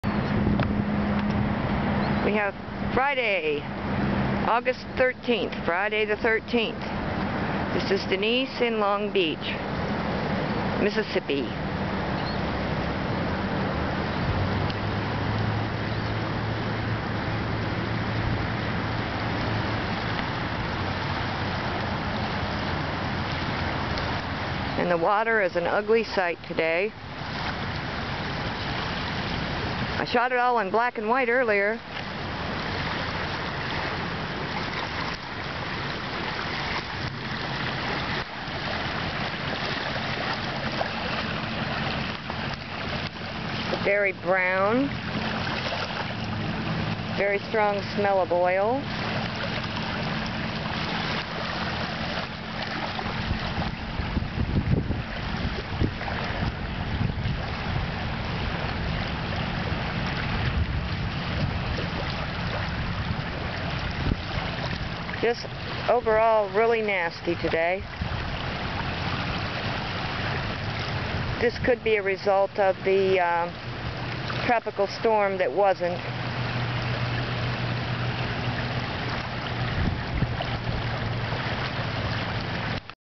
A Mississippi Mess of Huge Proportions - by Denise Rednour, August 13th
The water is completely saturated with dispersed oil and pockets of dead sea life lay on the bottom in areas along sea walls, etc.  The air still smells fowled with chemicals and petroleum and there is no sign whatsoever of any clean up attempts.